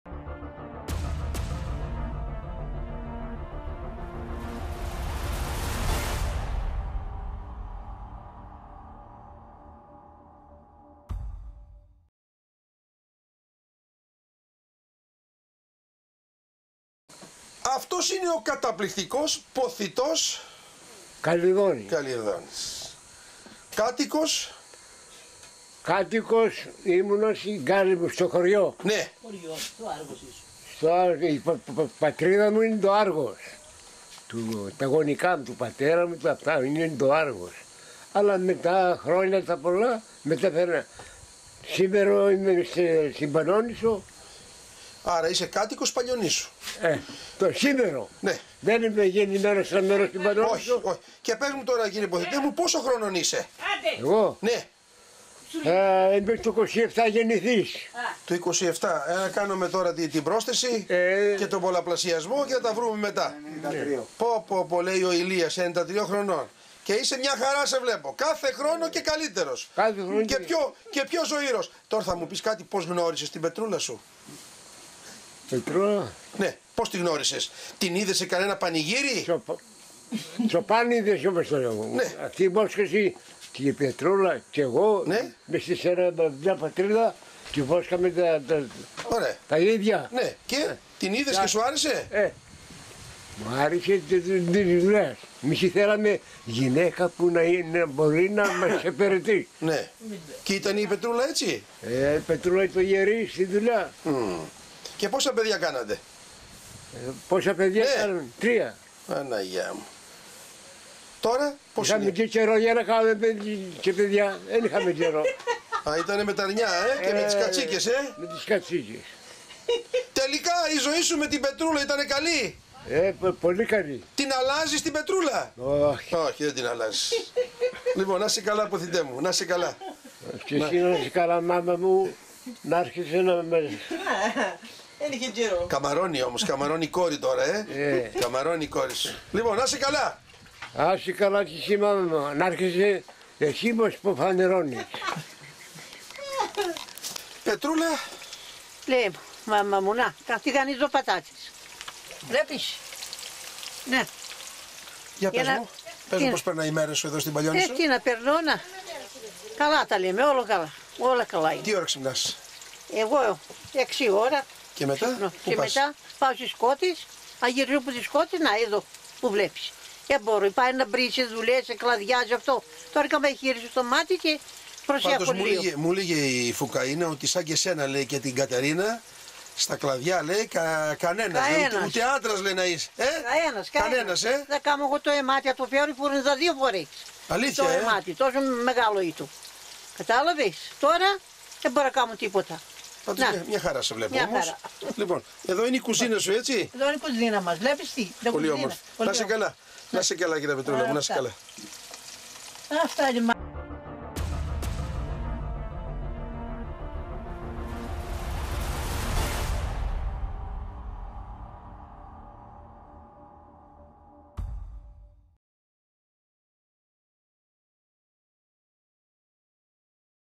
31-5-2018 Ποθητός και Πετρούλα Καλλιδώνη ένα ταιριαστό ζευγάρι της Παλιονήσου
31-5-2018   Ποθητός και Πετρούλα Καλλιδώνη ένα ταιριαστό ζευγάρι της Παλιονήσου